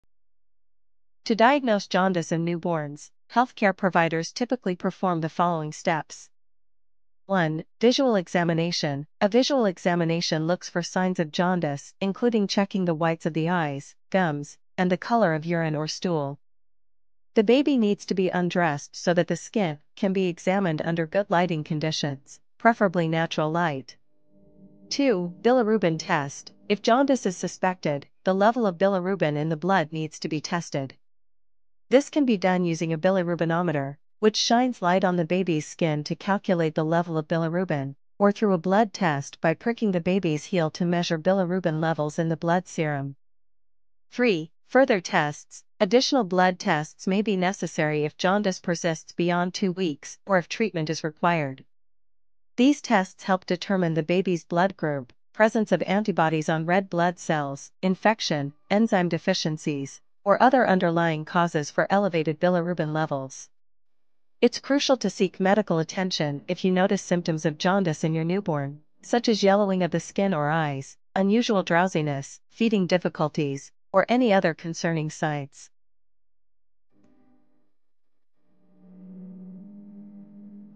How to Check Jaundice in Babies?
To diagnose jaundice in newborns, healthcare providers typically perform the following steps:

1. **Visual examination**: A visual examination looks for signs of jaundice  including checking the yellowness in the whites of the eyes, gums, and the color of urine or stool. The baby needs to be undressed so that the skin can be examined under good lighting conditions, preferably natural light.
2. **Bilirubin test**: If jaundice is suspected, the level of bilirubin in the blood needs to be tested. This can be done using a bilirubinometer, which shines light on the baby's skin to calculate the level of bilirubin, or through a blood test by pricking the baby's heel to measure bilirubin levels in the blood serum.
3. **Further tests**: Additional blood tests may be necessary if jaundice persists beyond two weeks or if treatment is required. These tests help determine the baby's blood group, presence of antibodies on red blood cells, infection, enzyme deficiencies, or other underlying causes for elevated bilirubin levels.

It's crucial to seek medical attention if you notice symptoms of jaundice in your newborn, such as yellowing of the skin or eyes, unusual drowsiness, feeding difficulties, or any other concerning signs.

Source1:-Newborn jaundice. (n.d.). Newborn jaundice.

Source2:-Newborn jaundice - Diagnosis. (n.d.). Newborn jaundice - Diagnosis.